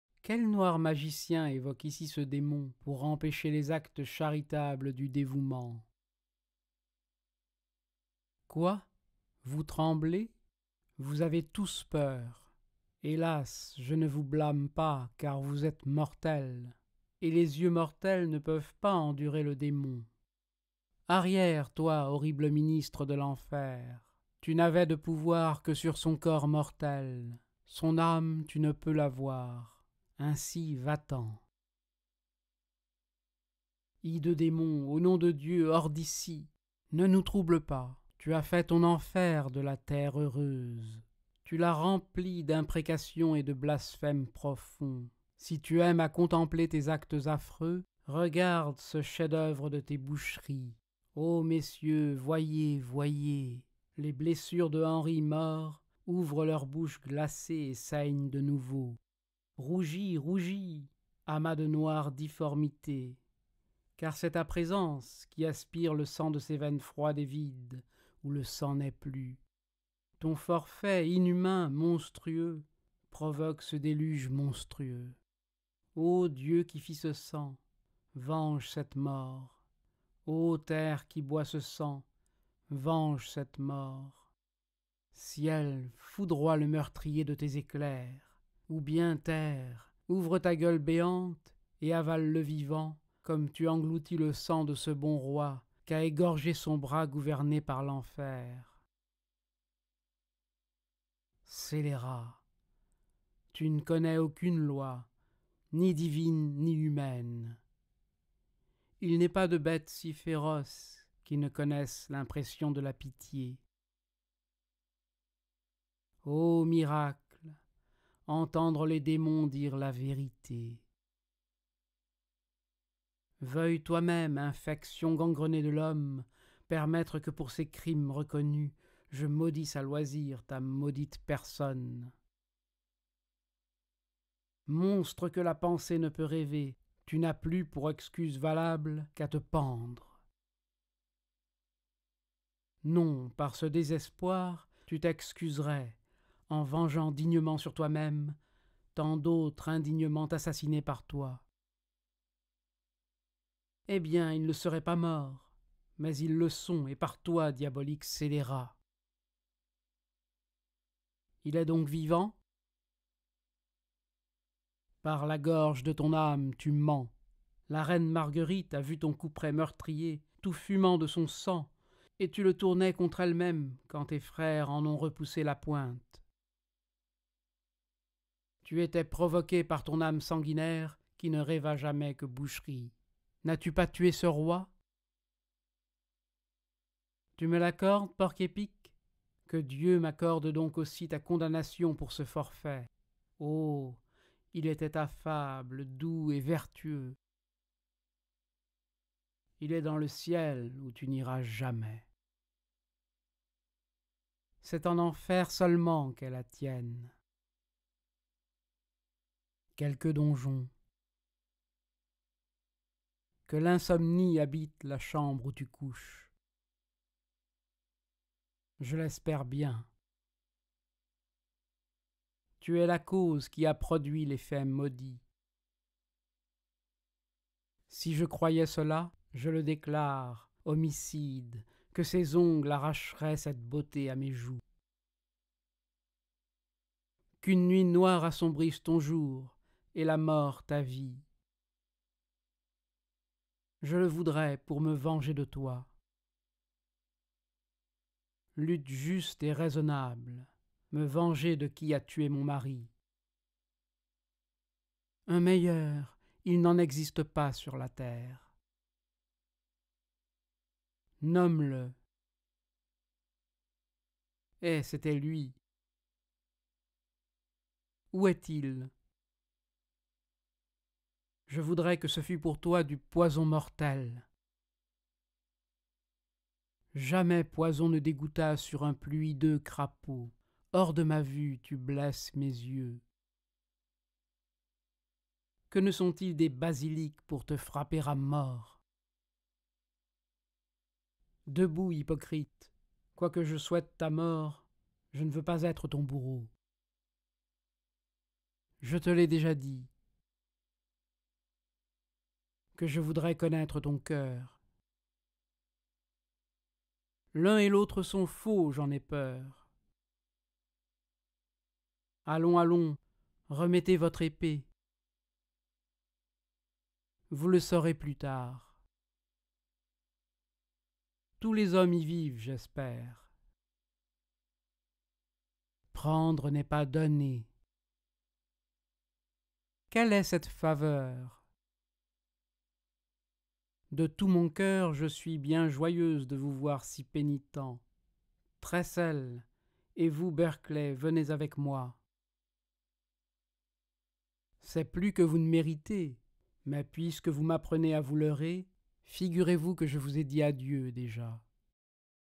Richard III, acte I, scène II, répliques de Lady Ann, Shakespeare, traduit par François-Victor Hugo
Quel noir magicien évoque ici ce démon pour empêcher les actes charitables du dévouement ?Quoi ! vous tremblez ? vous avez tous peur ? — Hélas, je ne vous blâme pas, car vous êtes mortels, — et les yeux mortels ne peuvent pas endurer le démon. — Arrière, toi, horrible ministre de l’enfer ! — Tu n’avais de pouvoir que sur son corps mortel. — Son âme, tu ne peux l’avoir. Ainsi, va-t’en !

— Hideux démon, au nom de Dieu, hors d’ici ! Ne nous trouble pas. — Tu as fait ton enfer de la terre heureuse. — Tu l’as remplie d’imprécations et de blasphèmes profonds. — Si tu aimes à contempler tes actes affreux, — regarde ce chef-d’œuvre de tes boucheries ! — Oh ! messieurs, voyez, voyez ! Les blessures de Henry mort — ouvrent leurs bouches glacées et saignent de nouveau (45) ! — Rougis, rougis, amas de noires difformités, — car c’est ta présence qui aspire le sang — de ces veines froides et vides où le sang n’est plus. — Ton forfait, inhumain, monstrueux, — provoque ce déluge monstrueux. — Ô Dieu, qui fis ce sang, venge cette mort ! — Ô terre, qui bois ce sang, venge cette mort ! — Ciel, foudroie le meurtrier de tes éclairs ; — ou bien, terre, ouvre ta gueule béante, et mange-le vivant ; — comme tu engloutis le sang de ce bon roi — qu’a égorgé son bras gouverné par l’enfer !

— Scélérat, tu ne connais aucune loi, ni divine, ni humaine : — il n’est pas de bête si féroce qui ne connaisse l’impression de la pitié.

— Ô miracle ! entendre les démons dire la vérité !
supposés.

— Veuille toi-même, infection gangrenée de l’homme, — permettre que, pour ces crimes reconnus, — je maudisse à loisir ta maudite personne.

— Monstre que la pensée ne peut rêver, tu n’as plus, — pour excuse valable, qu’à te pendre.

— Non ! par ce désespoir, tu t’excuserais, — en vengeant dignement sur toi-même — tant d’autres indignement assassinés par toi.

Eh bien, ils ne seraient pas morts ; — mais ils le sont, et par toi, diabolique scélérat !

Il est donc vivant ?

— Par la gorge de ton âme, tu mens ! La reine Marguerite a vu — ton couperet meurtrier tout fumant de son sang, — et tu le tournais contre elle-même, — quand tes frères en ont repoussé la pointe.

— Tu étais provoqué par ton âme sanguinaire — qui ne rêva jamais que boucheries ; — N’as-tu pas tué ce roi ?

— Tu me l’accordes, porc-épic ? Que Dieu m’accorde donc aussi — ta damnation pour ce forfait ! — Oh ! il était affable, doux et vertueux !

— Il est dans le ciel, où tu n’iras jamais.

— C’est en enfer seulement qu’est la tienne !

— Quelque donjon.

— Que l’insomnie habite la chambre où tu couches !

— Je l’espère bien.

— Tu es la cause qui a produit l’effet maudit.

— Si je croyais cela, je te déclare, homicide, — que ces ongles arracheraient cette beauté de mes joues.

— Qu’une nuit noire assombrisse ton jour, et la mort ta vie !

— Je le voudrais, pour me venger de toi.

— Lutte juste et raisonnable ! — Me venger de qui a tué mon mari !

— Un meilleur ! il n’en existe pas sur la terre.

— Nomme-le.

Eh ! c’était lui.

— Où est-il ?

— Je voudrais que ce fût pour toi du poison mortel !

— Jamais poison ne dégoutta sur un plus hideux crapaud. — Hors de ma vue ! tu blesses mes yeux.

— Que ne sont-ils des basilics pour te frapper à mort !

— Debout, hypocrite ! Quoique je souhaite ta mort, — je ne veux pas être ton bourreau.

— Je te l’ai déjà dit.

— Que je voudrais connaître ton cœur !

L’un et l’autre sont faux, j’en ai peur.

Allons, allons, remettez votre épée.

Vous le saurez — plus tard.

Tous les hommes — y vivent, j’espère.

— Prendre n’est pas donner.

— Quelle est cette faveur ?

— De tout mon cœur ; je suis bien joyeuse — de vous voir devenu si pénitent. — Tressel, et vous, Berkley, venez avec moi.

C’est plus que vous ne méritez, — mais, puisque vous m’apprenez à vous leurrer, — figurez-vous que je vous ai dit adieu déjà.

– Activez les sous-titres dans la vidéo pour suivre avec le texte original –

Lu par Michel Garçon, professeur de phonétique à Paris et par Skype

Richard III, acte I, scène II, répliques de Lady Ann, Shakespeare
traduit par François-Victor Hugo